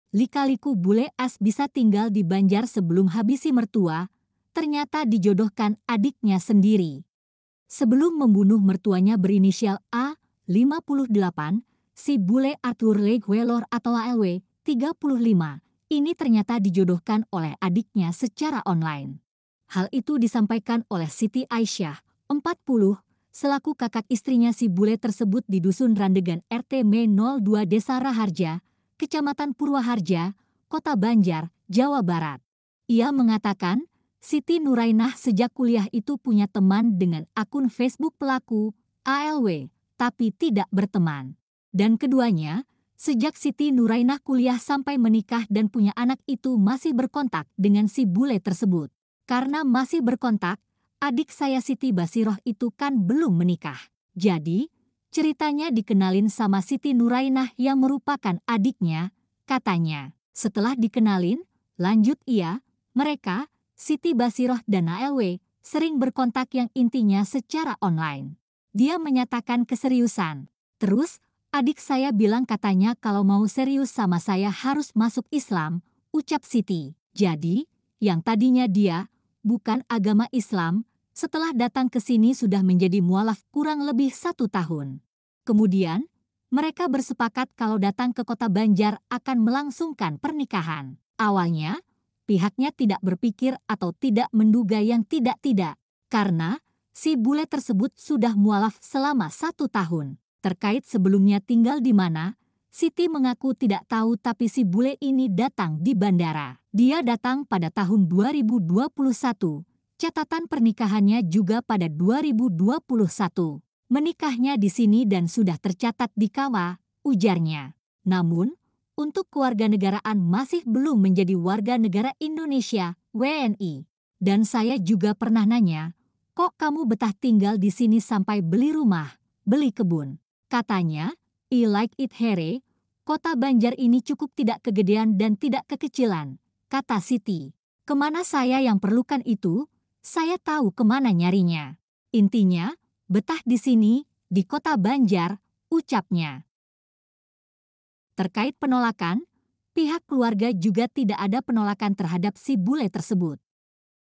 LIKA-LIKU Bule AS Bisa Tinggal di Banjar sebelum Habisi Mertua, Ternyata Dijodohkan Adiknya Sendiri
Sebelum habisi mertuanya berinisial A (58), si bule Arthur Leigh Welohr atau ALW (35) ini ternyata dijodohkan oleh adiknya secara online.